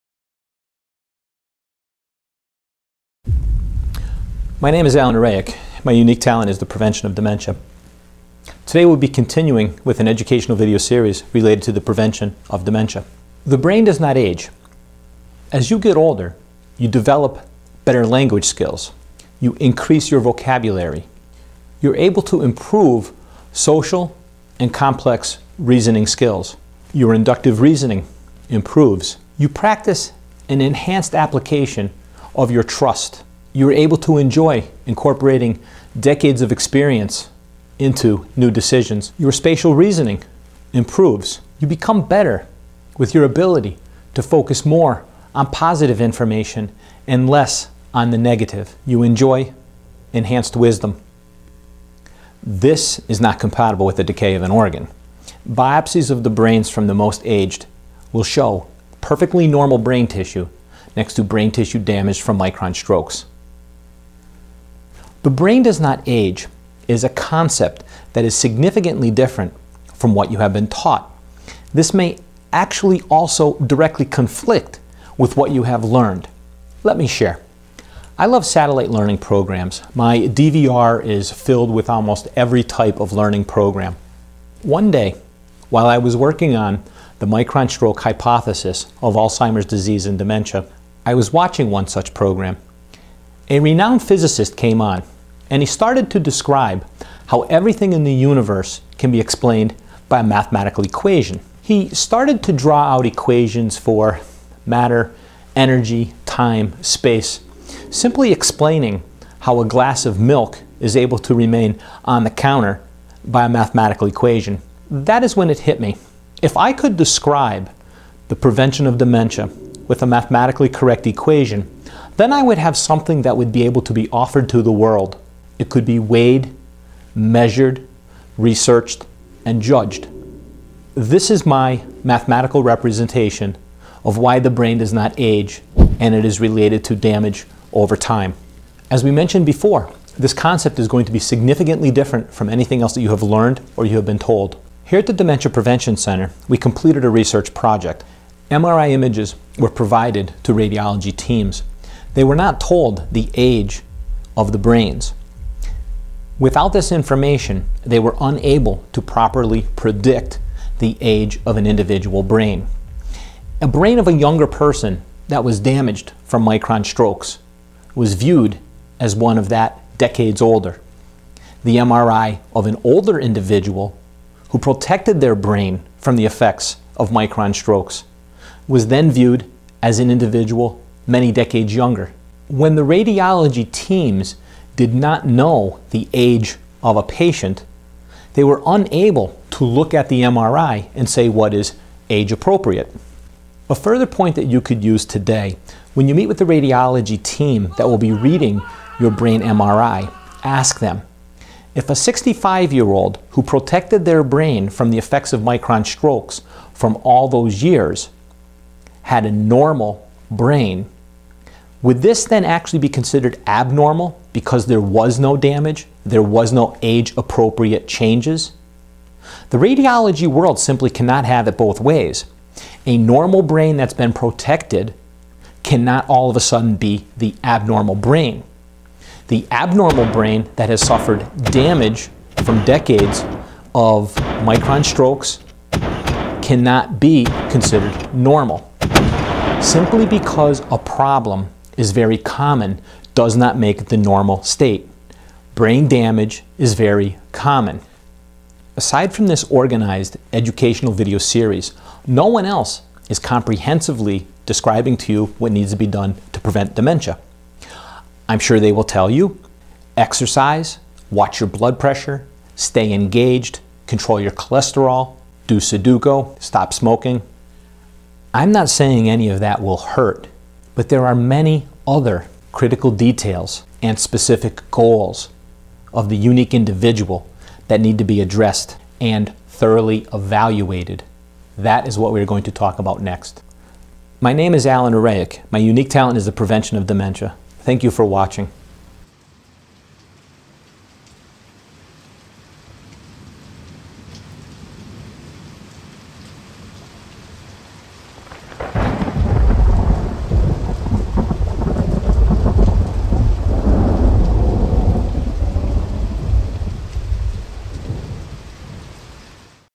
Physician explains method to prevent Alzheimer's and Dementia - Part 5/6. The brain does not age.
The Dementia Prevention Center and Allen J. Orehek, M.D. (innovator & physician)present part 5 of a video series on how to prevent dementia. In this video the explanation of why the brain does not age - there is damage over time that occurs.  Damage to the brain is common in the current world, but is not the normal state - learn how to protect your brain and thus your mind/memory.